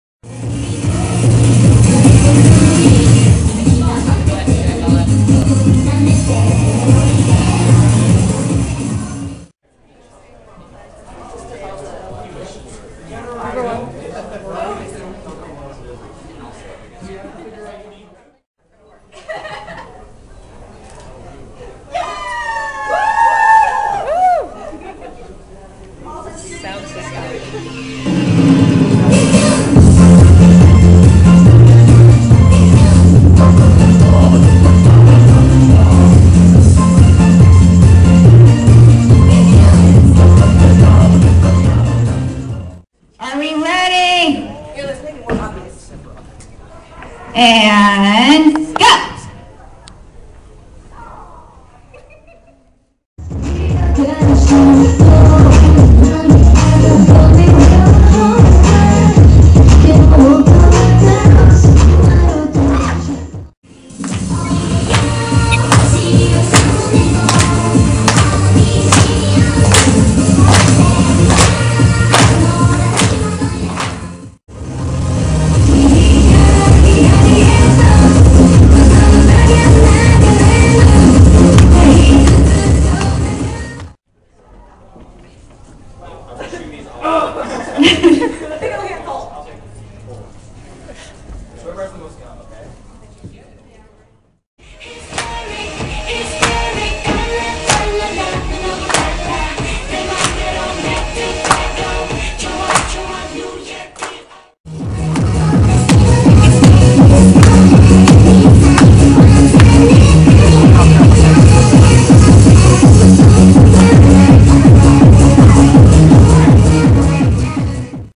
Team Moe Kira Kira Special Preview
We were super excited to do this performance and even with last minute problems including the sound system we made it through!  Here is a quick preview while I edit the rest of the videos.